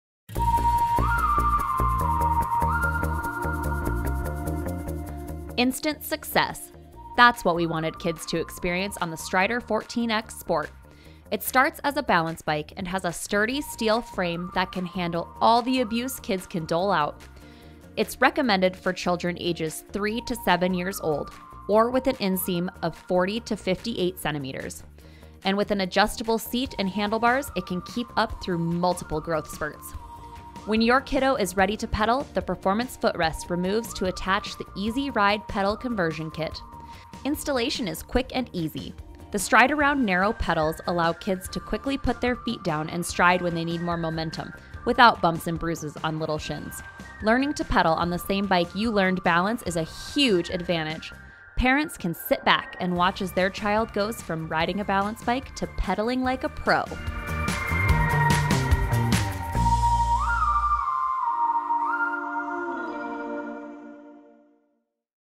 Strider 14x Sport Product Overview
Instant success. That’s what we wanted kids to experience on the Strider 14x. It starts as a balance bike and has a sturdy, steel frame that can handle all the abuse kids can dole out. It is recommended for children ages 3-7 or with an inseam of 16-23 inches (40-58cm), and with an adjustable seat and handlebars it can keep up through multiple growth spurts. When your kiddo is ready to pedal, the performance footrest removes to attach the Easy-Ride Pedal Conversion Kit. Installation is quick and easy. The Stride-Around narrow pedals allow kids to quickly put their feet down and stride when they need more momentum without bumps and bruises on little shins. Learning to pedal on the same bike you learned balance is a HUGE advantage. Parents can sit back and watch as their child goes from riding a balance bike to pedaling like a pro.